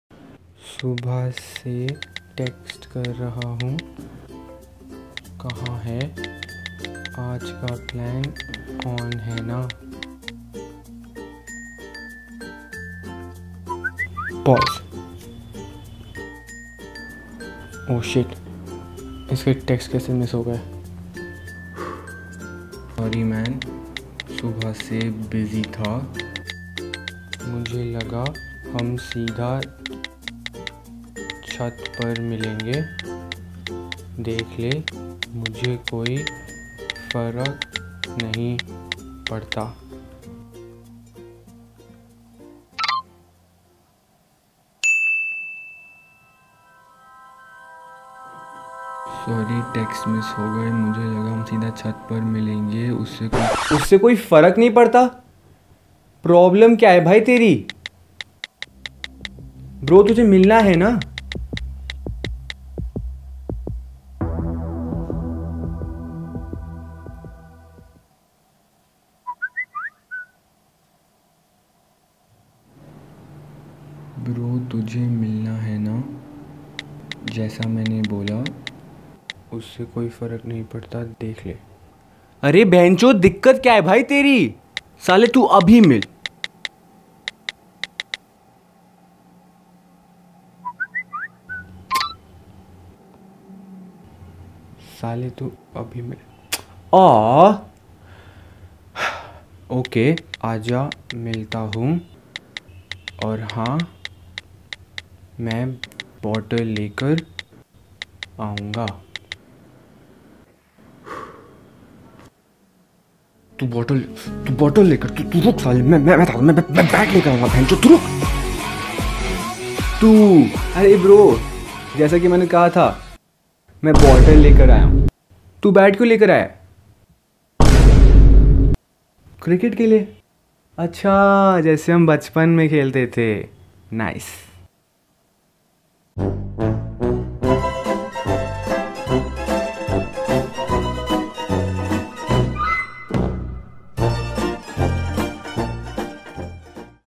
WHEN YOU INTERPRET TEXTS WRONGLY
People Misunderstand texts on a daily basis, see what happens when manav misunderstands some very angry texts.